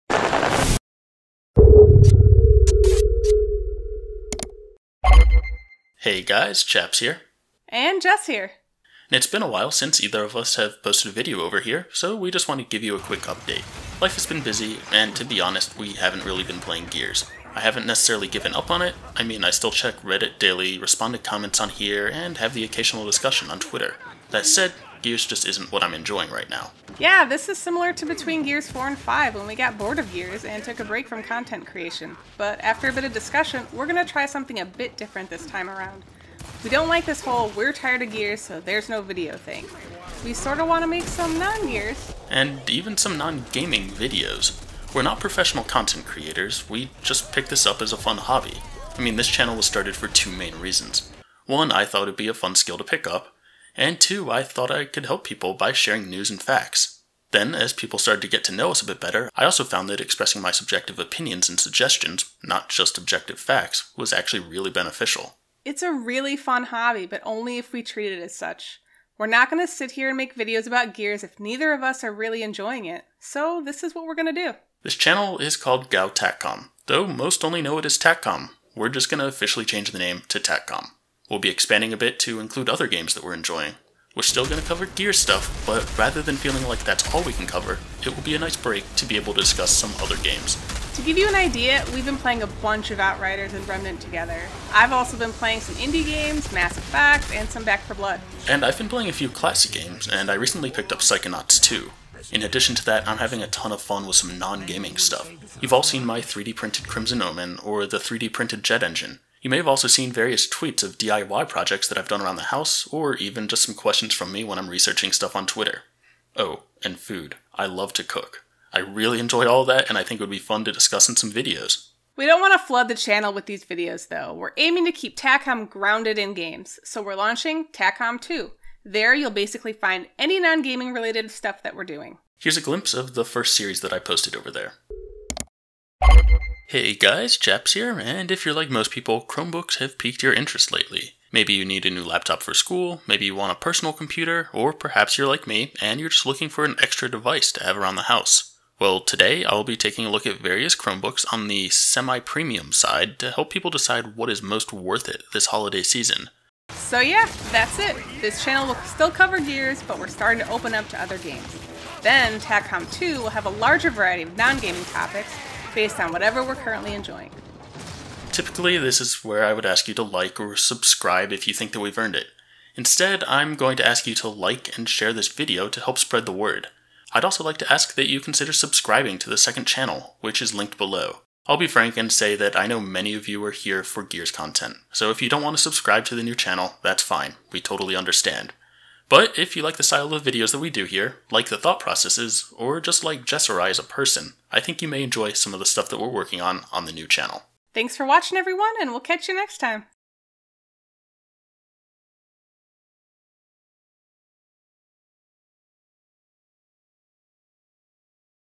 Tac-Com: What's Next
What's next for Tac-Com?
You may have noticed that we haven't posted any videos recently. Well, truth be told, we're not really enjoying Gears at the moment. But we don't want that to stop of from video editing, and there's probably other content that you'd enjoy. Because of that, we're making some changes:
- Tac-Com - All gaming content (not just Gears)
- Tac-Com 2 - Non-gaming content